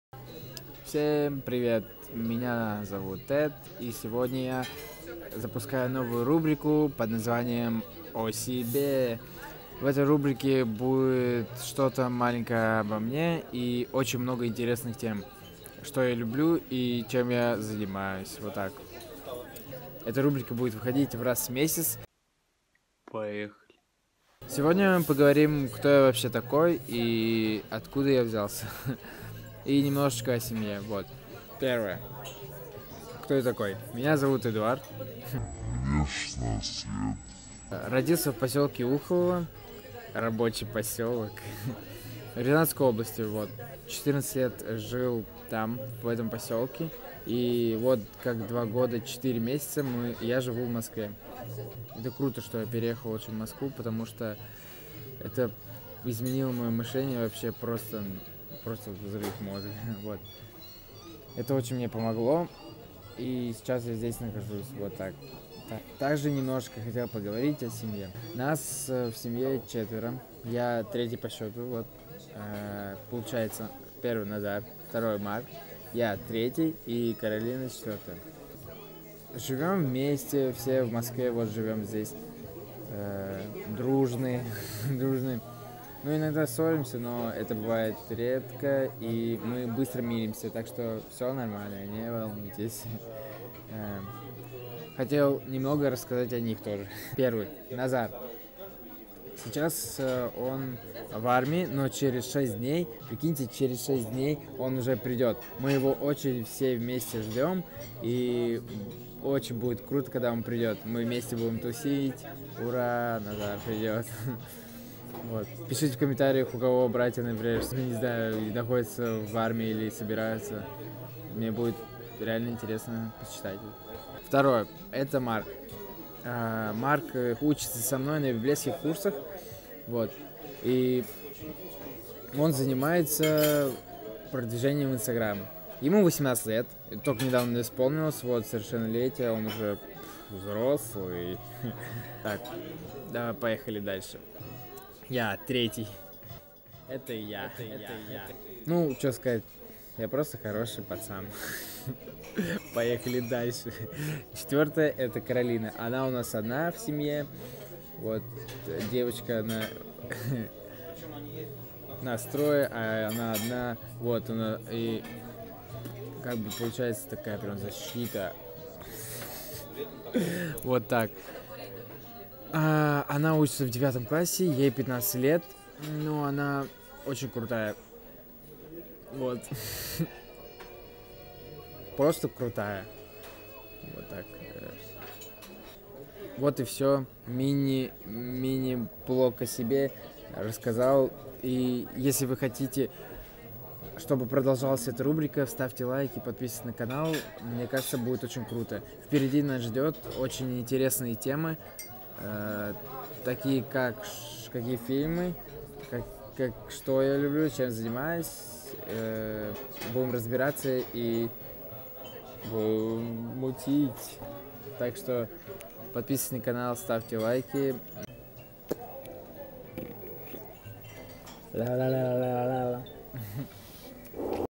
| НОВАЯ РУБРИКА | О СЕБЕ | И МНОГОЕ ДРУГОЕ |
Ребята всем привет меня зовут ЭД
Запускаю новую Рубрику & О СЕБЕ &
Данная рубрика будет выходить раз месяц)

В дальнейшем будет все интереснее и интереснее , так что не уходи , будет круто , я отвечаю.

по братски

А ТАК ЖЕ МОИ СОЦ.СЕТИ